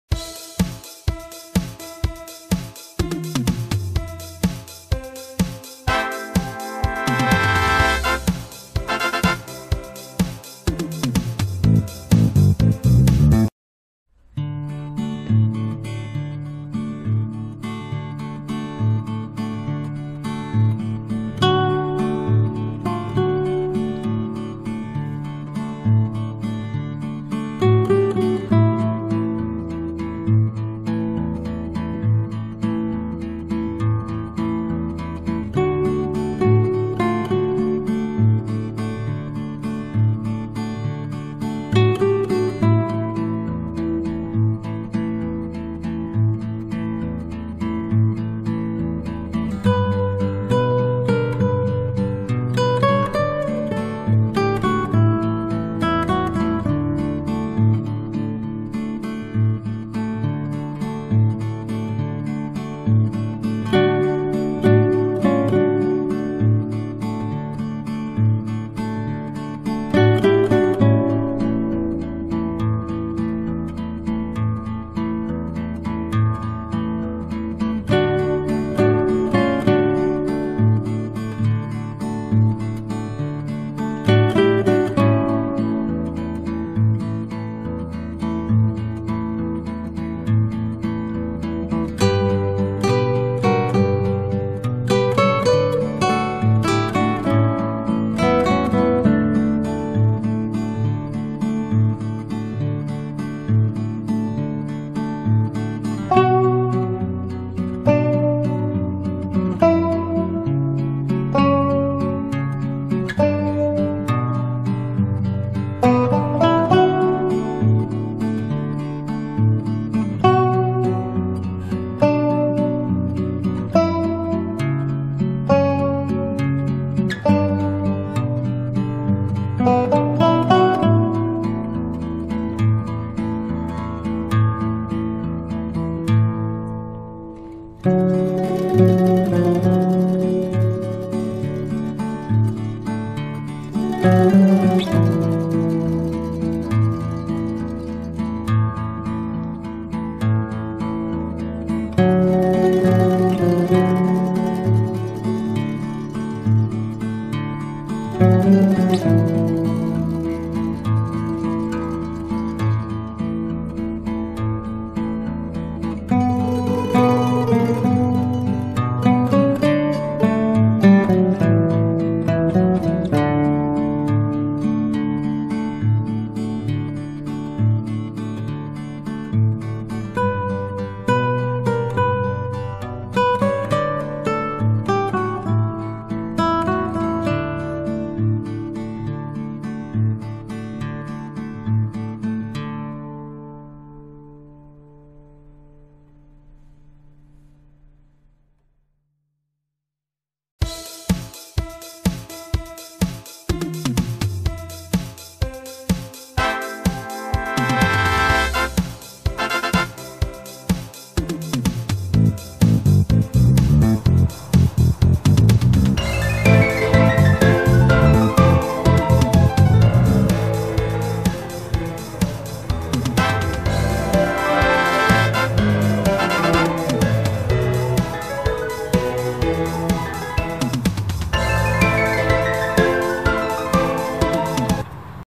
345 OLYMPUS📷E-M10 MARK III HIGH-SPEED TESTING AND SAMPLE PHOTOS UHD 4K/Quay phim chậm, và hình
Handheld and some photos shooting with backlight HDR and Art mode (Pop-Art II. High-Speed movie with 120fr per second, 14-42mm lens,@30p
Cầm máy bằng ✋ chụp hình bằng hệ HDR ánh sáng phía sau và cho vào hệ lược màu trong máy.
Quay chậm bằng hệ 120 khủng cho 1 giây với ống kiếng 14-42 ly, tốc độ 30p.
Camera we used:
Pentax K-50 18-55mm, 55-200mm
Canon HF-R600 Camcoder
Ricoh Theta 360
Olympus OM-D E-M10 MK III 14-45mm